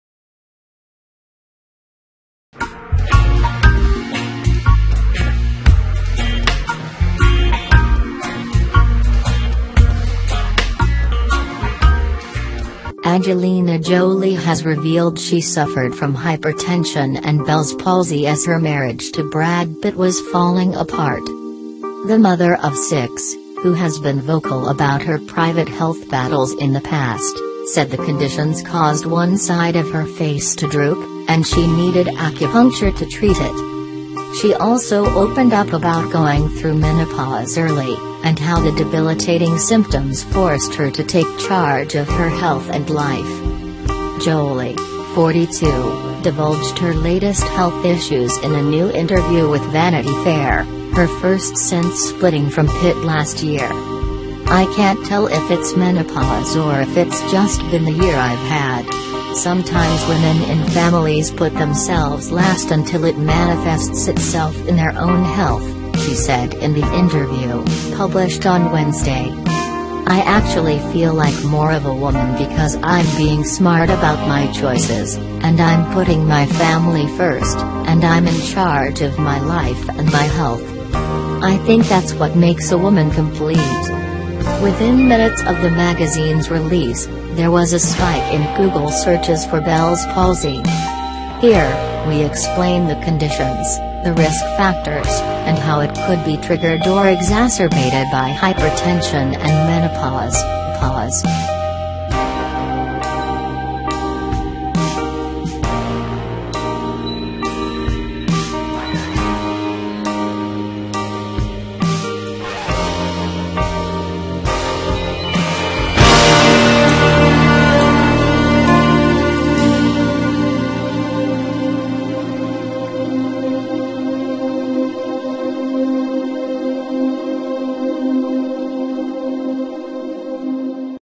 Angelina Jolie reveals "she had Bell s palsy this year"
Angelina Jolie reveals she suffered facial paralysis during her break-up from Brad Pitt